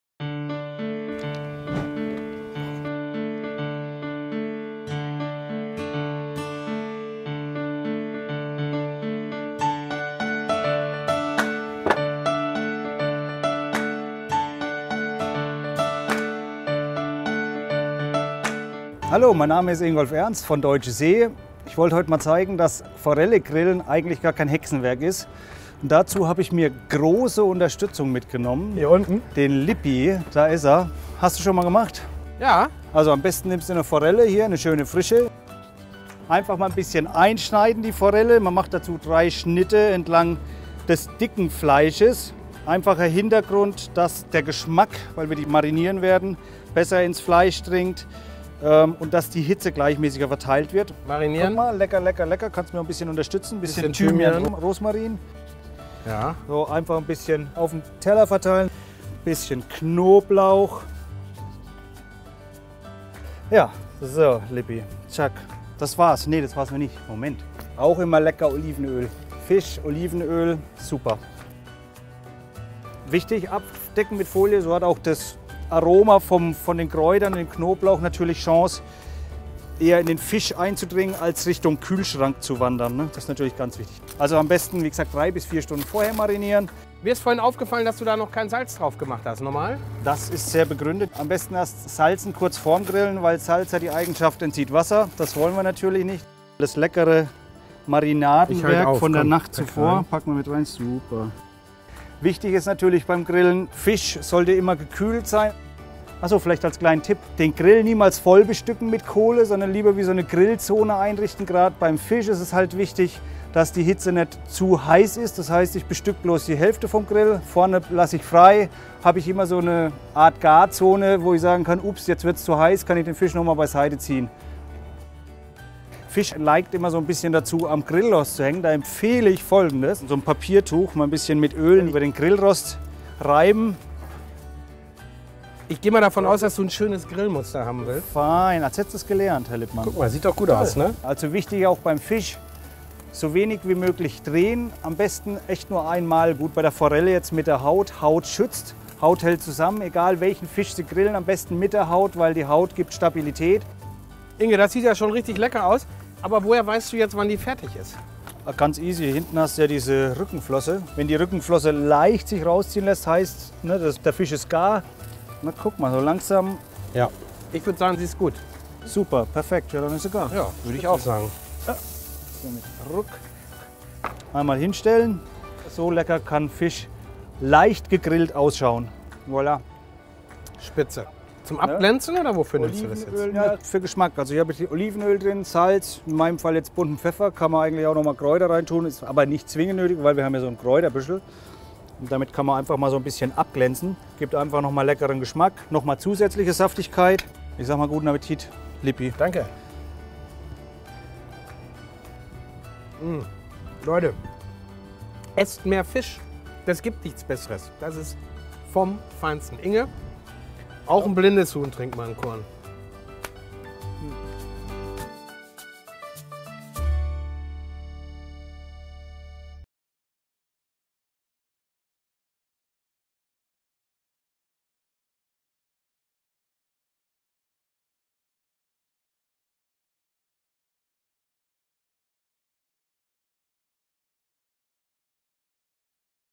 Hier grillt der Chef: 9 Tipps für Forelle
Forelle ist der ideale Grillfisch. Erfahren Sie von unserem Koch Ingolf Ernst, wie Sie Forelle perfekt grillen. Tatkräftig unterstützt von Andreas Lippmann verrät er Ihnen seine Fisch-Grill-Tricks. finden Sie die einzelnen Schritte in Bildern erklärt.

Dir hat das Video gefallen? Dann zeig es mit einem - "Mag ich"! 👍